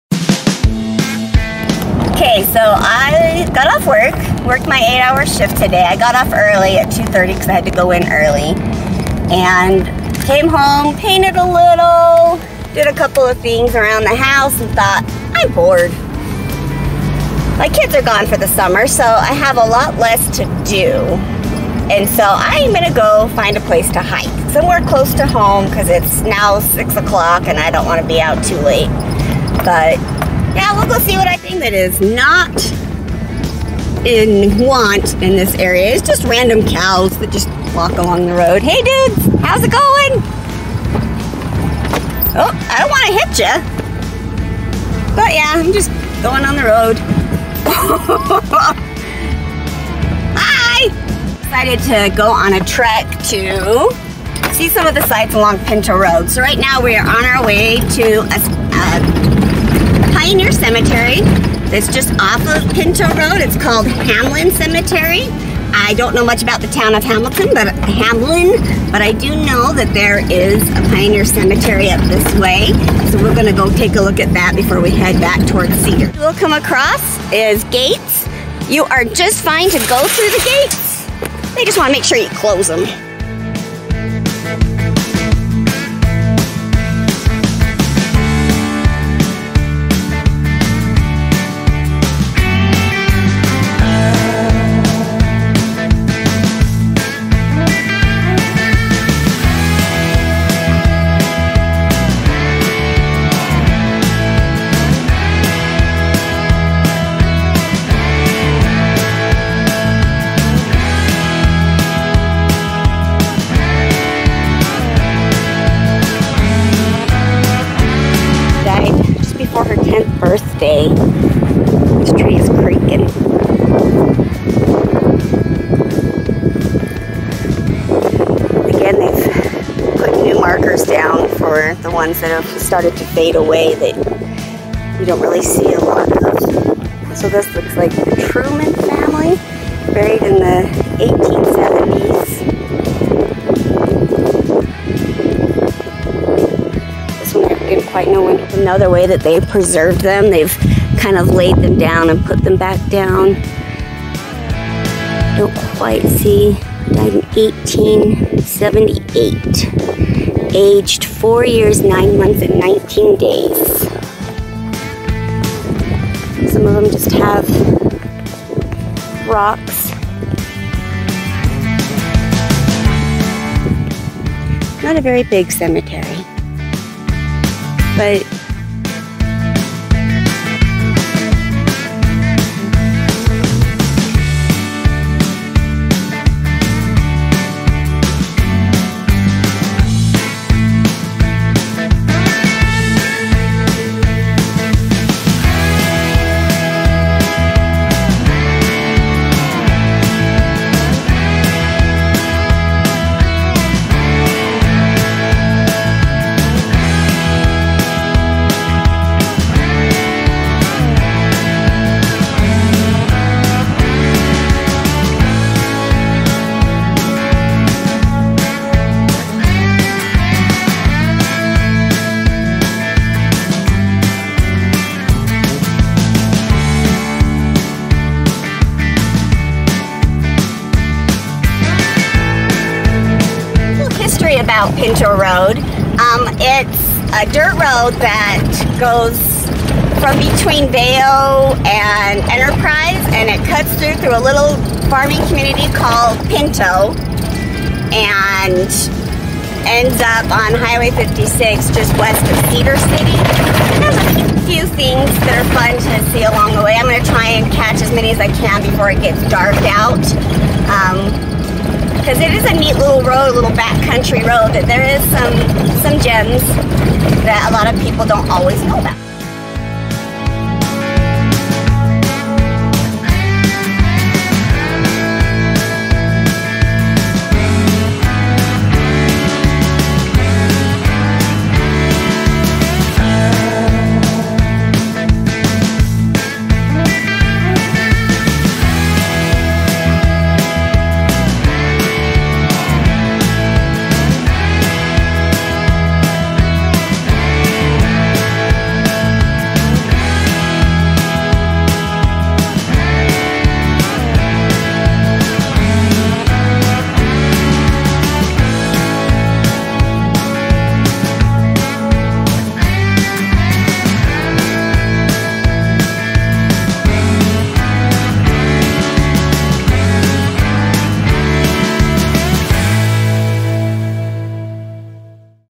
Exploring Pinto Road and Lion's Den
Pinto Road has some neat things to explore from a couple of pioneer cemeteries to an old pioneer homestead called Page Ranch. There is a neat Spring area and the nearest area includes Native American pictographs, or paintings, on the rocks.